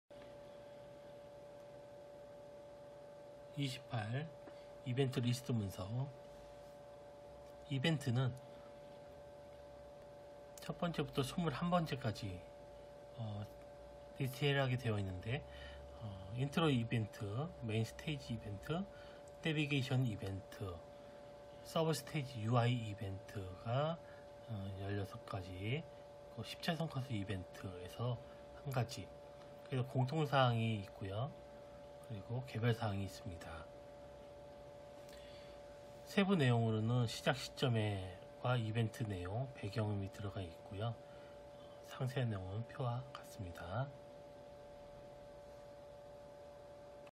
문화창조]39. VR게임 기획서 - 별첨8-28. 세부리스트 작성단계_이벤트 리스트문서
VR게임개발을 하며 작성한 문서들입니다.
제목은 Hi Dino(안녕 공룡)입니다.
1인개발 VR게임을 만들고 싶어 기획과 프로그램, 디자인까지 진행하였습니다.